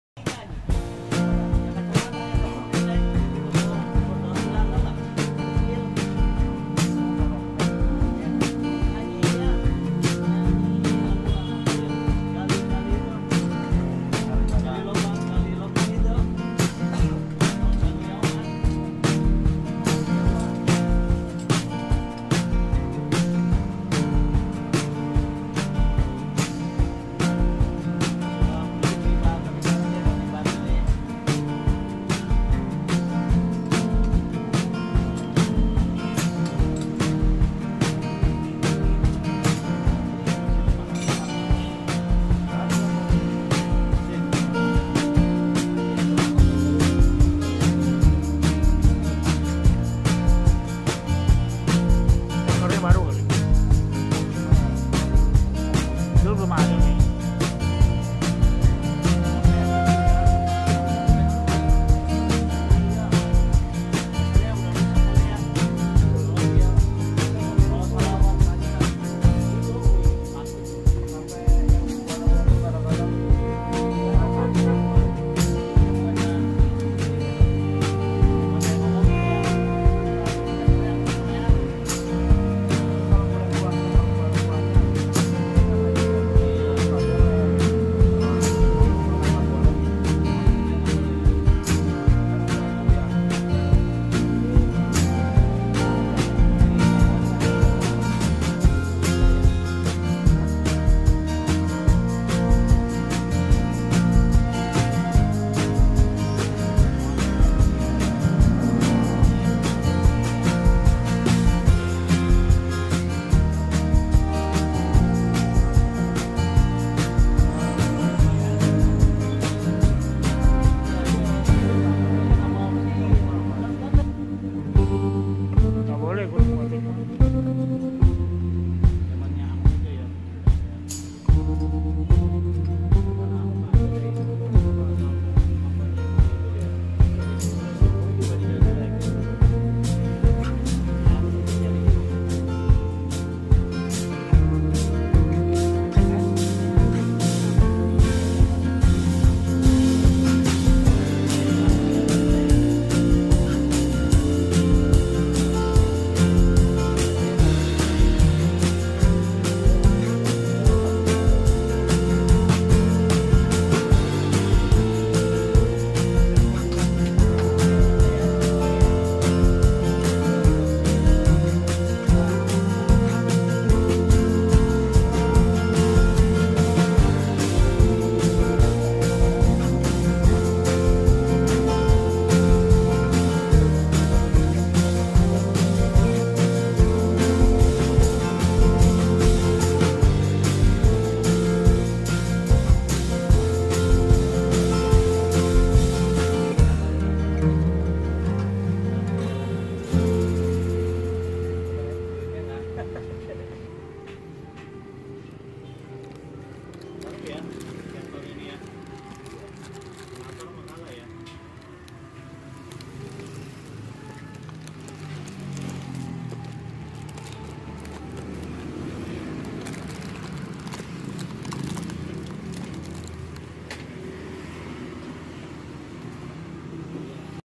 AVIATOR | Cara Merakit Sepeda Gunung | How to build a Bicycle
Cara merakit sepeda Gunung MTB 26" inch keluaran pabrik Pacific Indonesis.

Terima Kasih